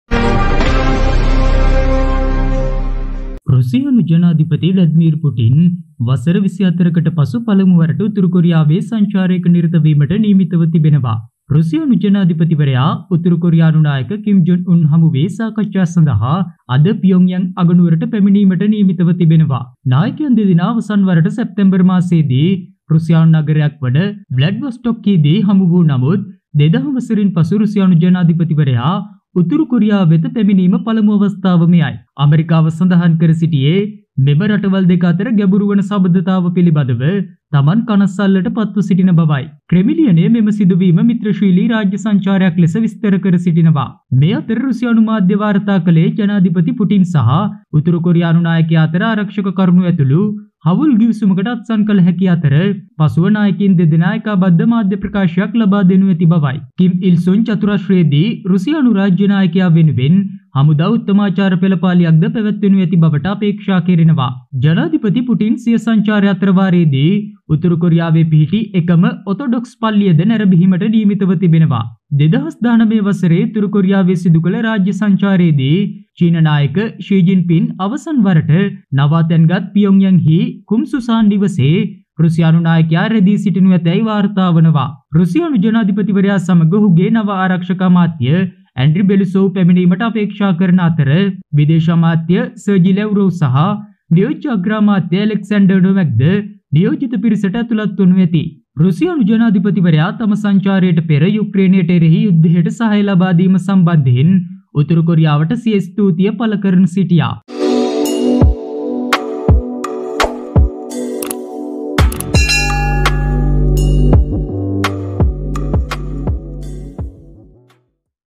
වසර 24කට පසුව පුටින් ගත් තීරණය | Russian President Vladimir Putin
Russian President Vladimir Putin is to visit North Korea for the first time in 24 years on Tuesday.
Mr Putin will meet North Korean leader Kim Jong Un for talks in the capital, Pyongyang.
The two leaders last met in September in the Russian city of Vladivostok but this is Mr Putin’s first visit to Pyongyang since 2000.
The United States said it was concerned about the "deepening relationship between these two countries".
The Kremlin has described the event as a “friendly state visit” with Russian media reporting Mr Putin and Mr Kim may sign a partnership agreement, including on security issues, and will give joint statements to the media.
A parade in Kim Il Sung square is anticipated. Mr Putin is also expected to watch a concert and visit the Orthodox Church of the Life-Giving Trinity in Pyongyang, the only orthodox church in North Korea.
There are reports Mr Putin will stay at the Kumsusan guesthouse in Pyongyang, where Chinese leader Xi Jinping last stayed during his state visit to North Korea in 2019.
Mr Putin is expected to arrive with his new defence minister, Andrei Belousov, while Foreign Minister Sergei Lavrov and Deputy Prime Minister Alexander Novak will also be part of the delegation.
The Russian president praised North Korea for "firmly supporting" Moscow's war in Ukraine ahead of his visit.